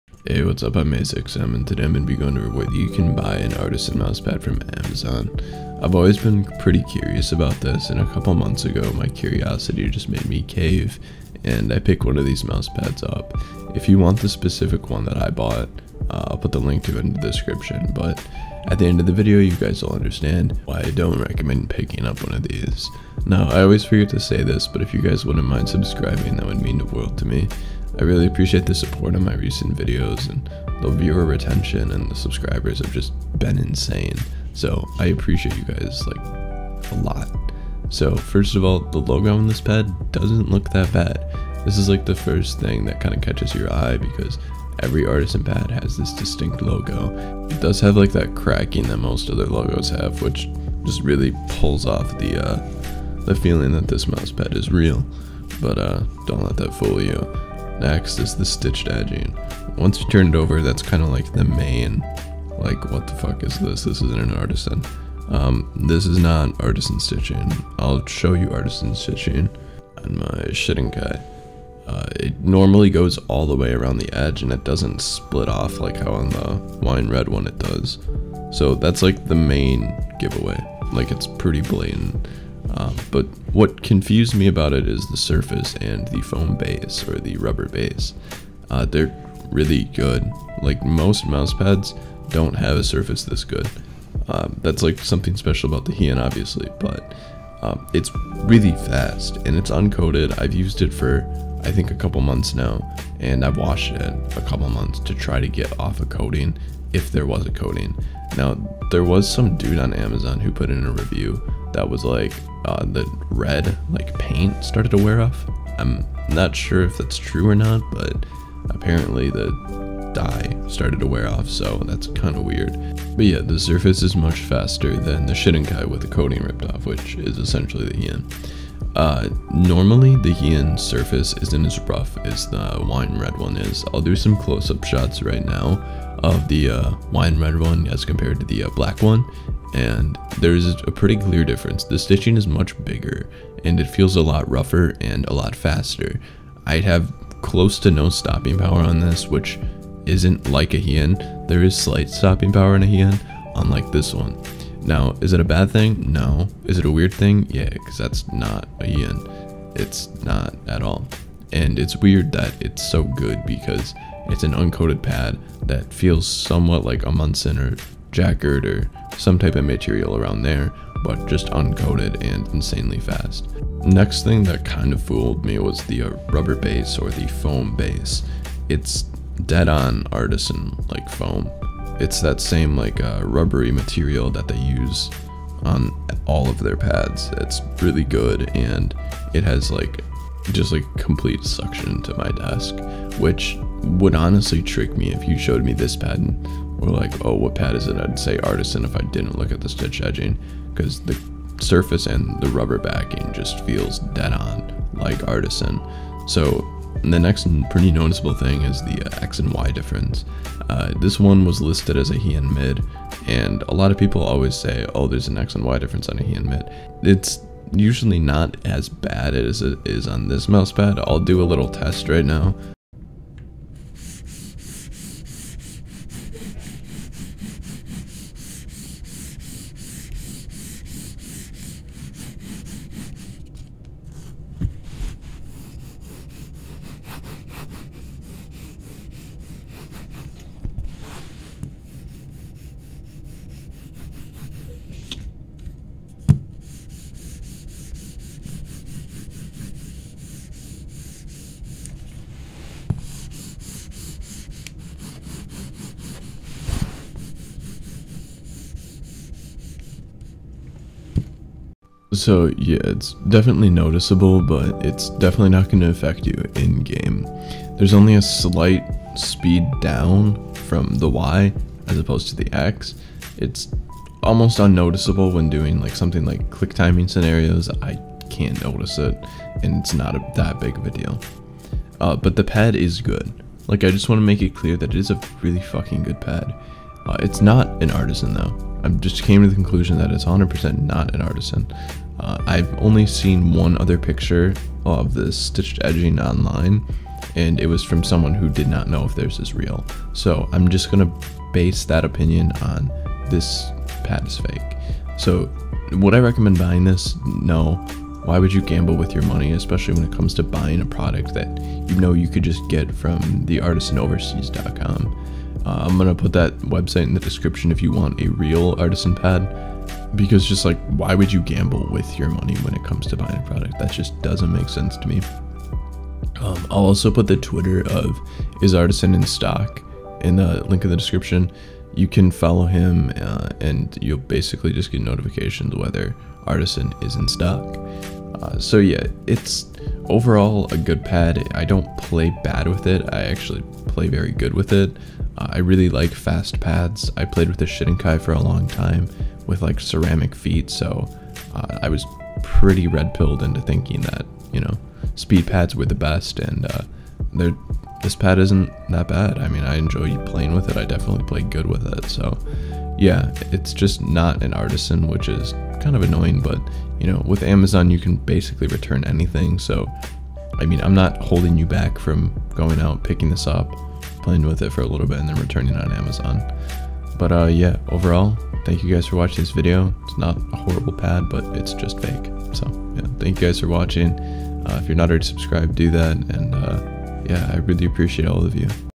You Can Buy REAL Artisan Mousepads On Amazon????
If this pad is real it has unacceptable quality.
Fake pad